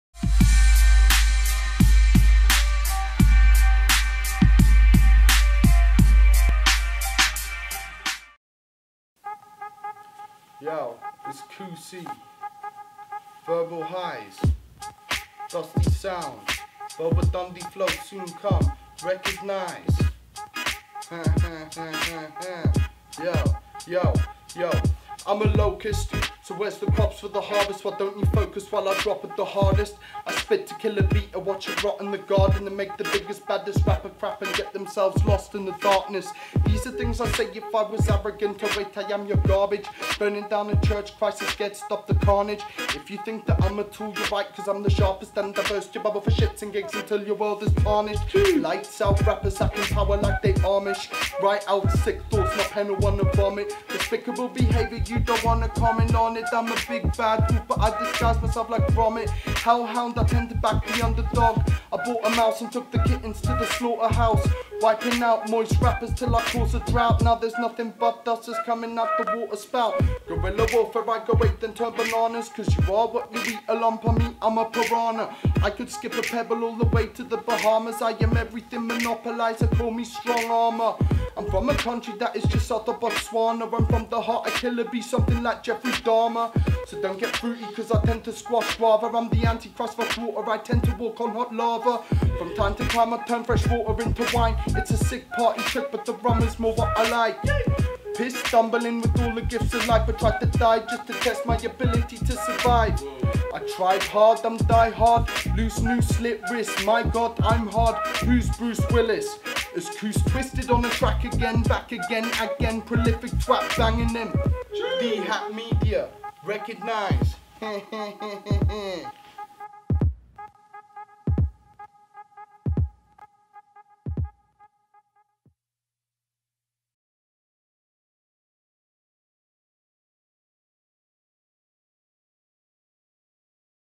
Coosie - [EXCLUSIVE BARS]
We linked up with Coosie of Verbal Highz to bring to you his debut exclusive bars.

Coosie/Verbal Highz -